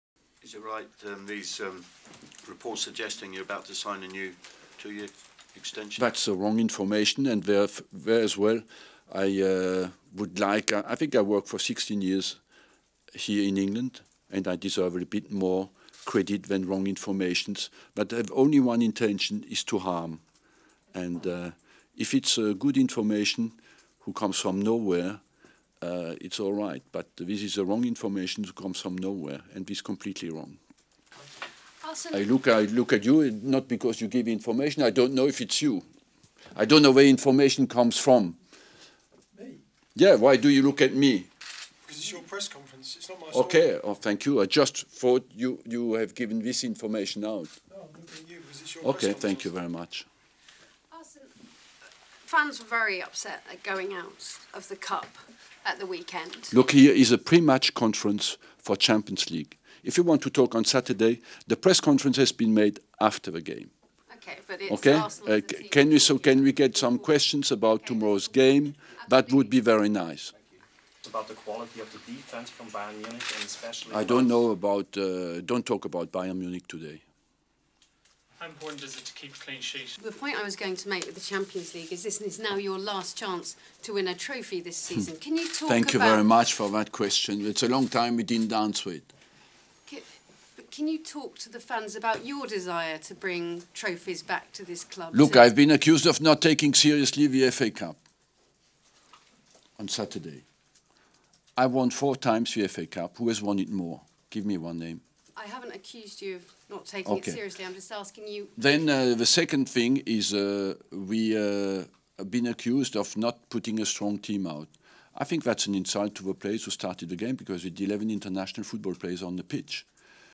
Arsene Wenger goes CRAZY at journalists
Arsene Wenger turns on journalists ahead of Arsenal's Bayern test.